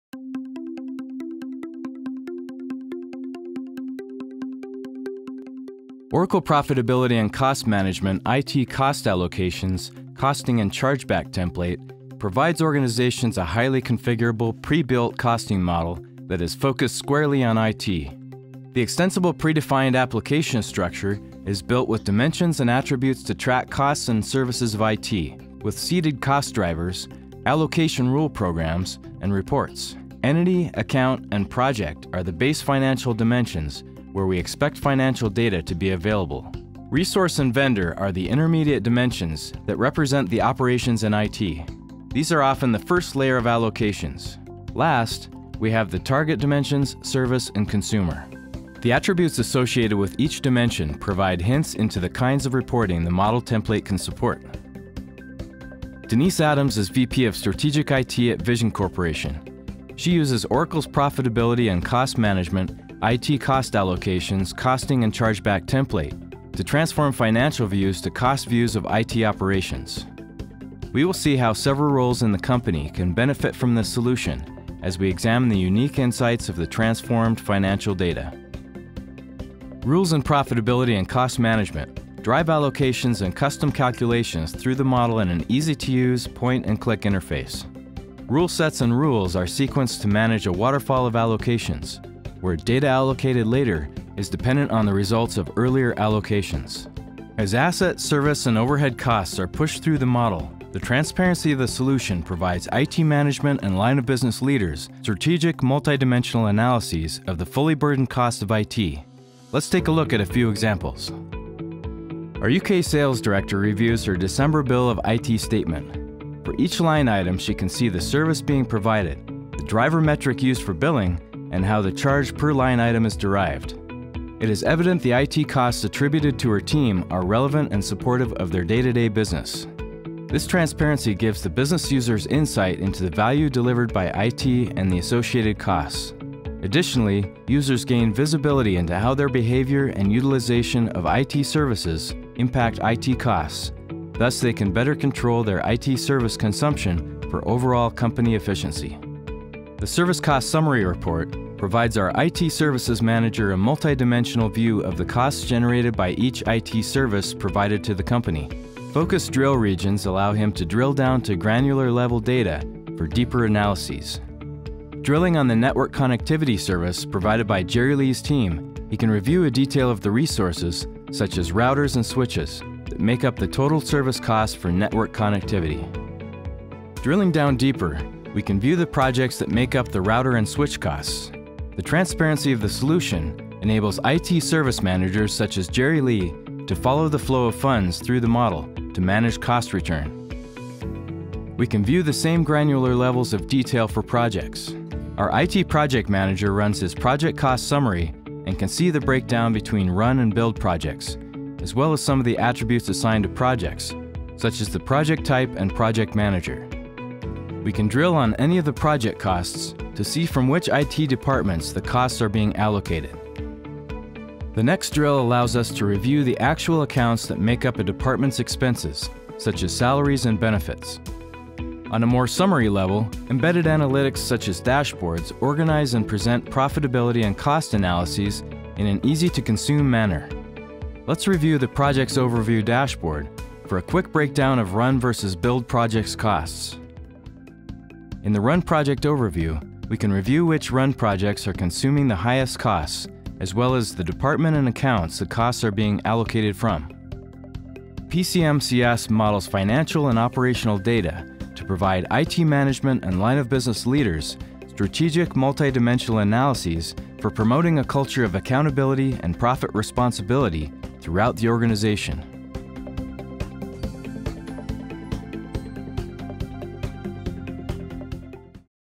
ITFM Cost Allocations
Oracle Profitability and Cost Management IT Cost Allocations Costing and Chargeback Template provides organizations a highly configurable prebuilt costing model that is focused squarely on IT.  Let's see how.